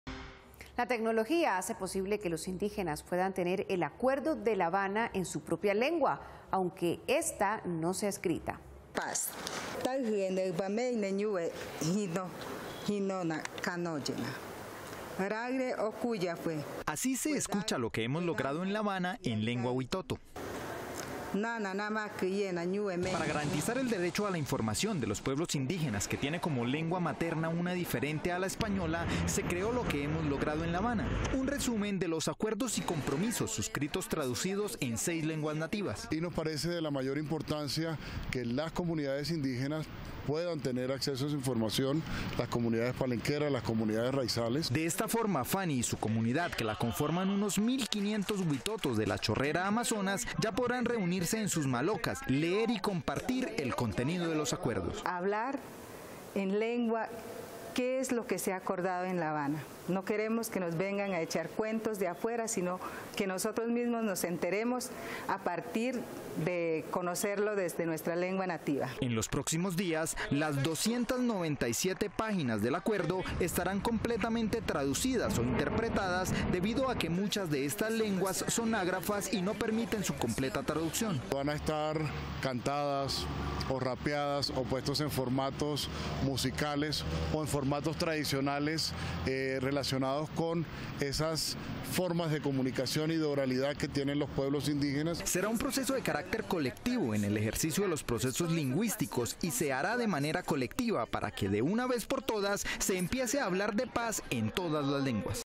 Los acuerdos de La Habana en lengua indígena
La tecnología hace posible que los indígenas puedan tener el acuerdo de La Habana en su propia lengua, aunque ésta no sea escrita.